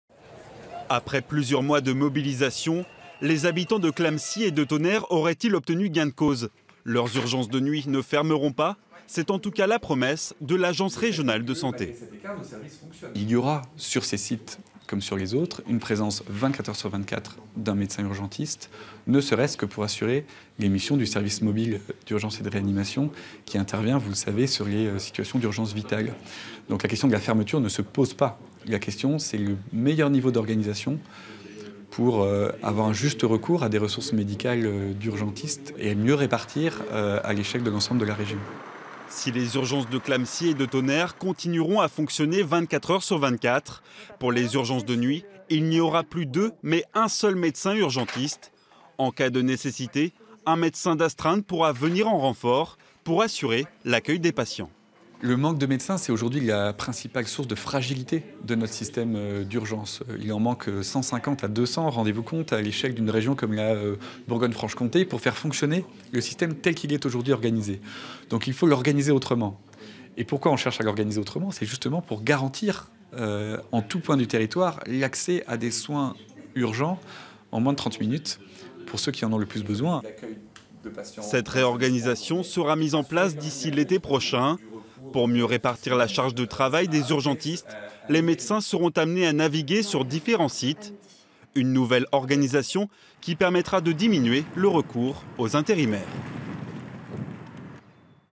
Les urgences de nuit de Tonnerre et de Clamecy ne fermeront pas
L’Agence régionale de santé (ARS) de Bourgogne-Franche-Comté a annoncé mercredi 4 avril que les urgences de nuit de Tonnerre (Yonne) et de Clamecy (Nièvre) ne fermeront pas.

Sébastien KERROUX, Christophe GAILLARD et Carlos ZAPPALÁ